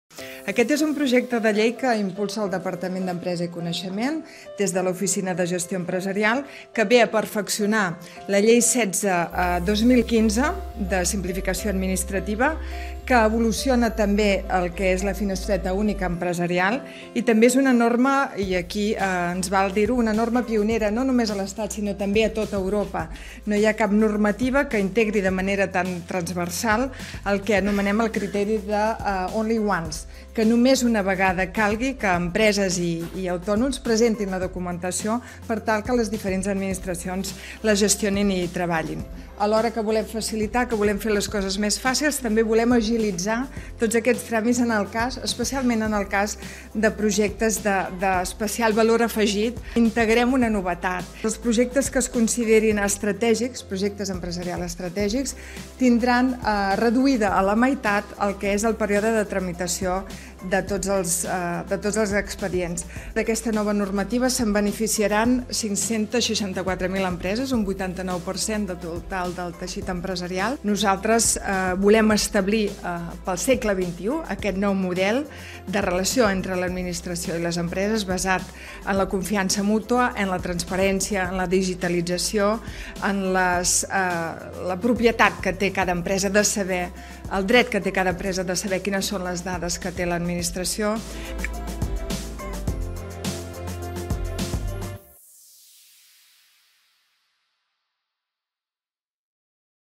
Coneix avantprojecte de llei de facilitació econòmica
El Govern aprova l'avantprojecte de llei de facilitació econòmica. La iniciativa, impulsada pel departament d'Empresa i Coneixement a través de l'Oficina de Gestió Empresarial, pretèn reduir al màxim els tràmits administratius vinculats a l'obertura d'un negoci.